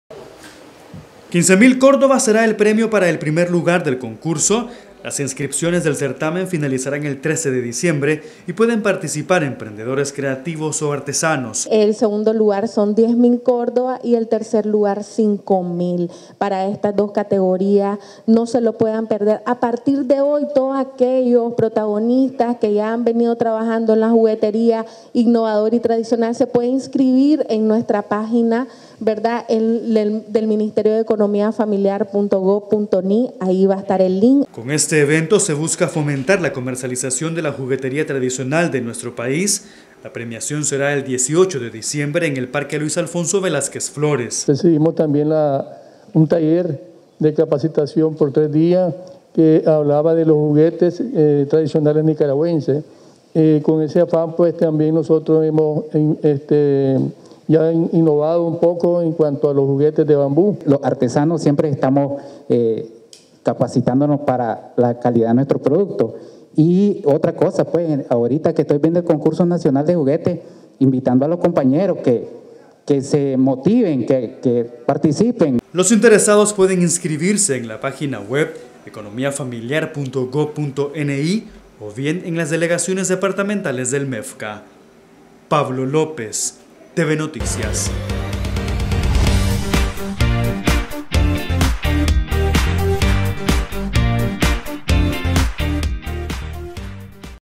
Abren Concurso Nacional de Juguetería tradicional nicaragüense
Fomentar el trabajo de los pequeños artesanos y emprendedores, es el objetivo del Concurso Nacional de Juguetería Tradicional Nicaragüense, el primer lugar obtendrá 15 mil córdobas, el segundo 10 mil y el tercero 5 mil